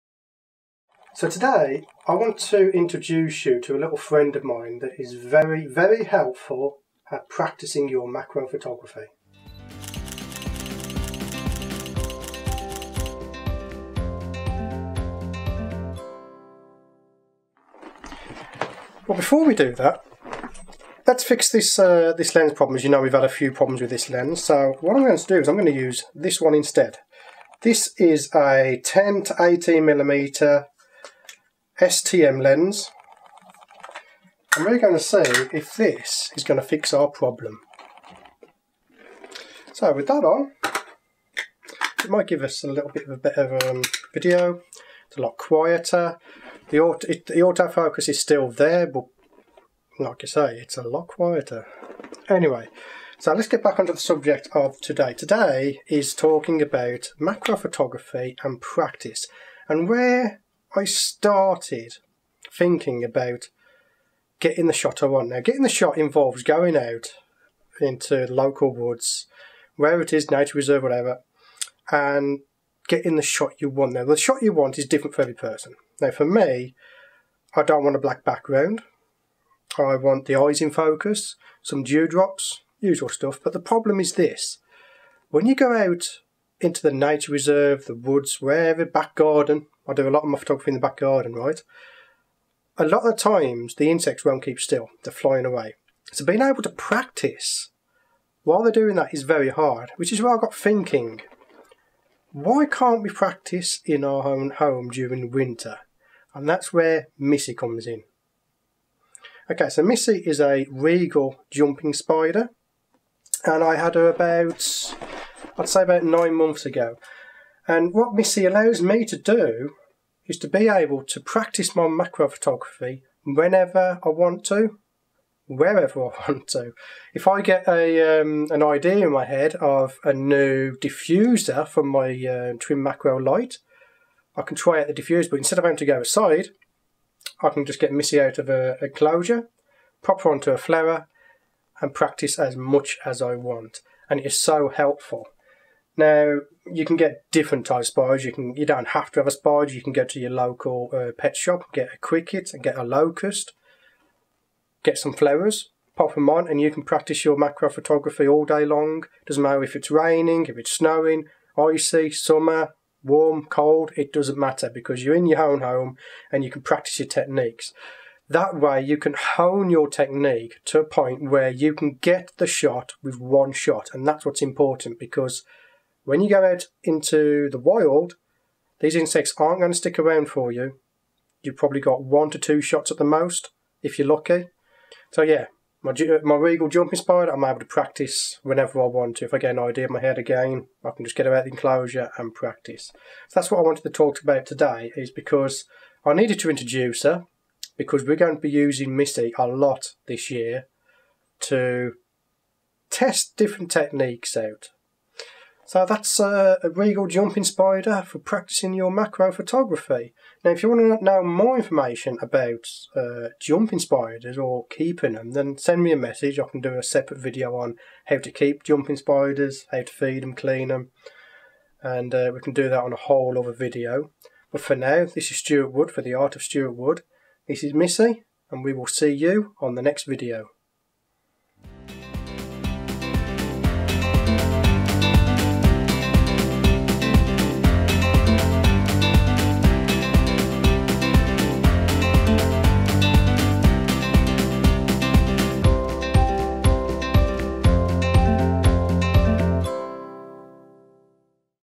Learning Macro photography with jumping spiders
In this video I want to talk about how you can practice your macro photography without the need to go outside and look for insects.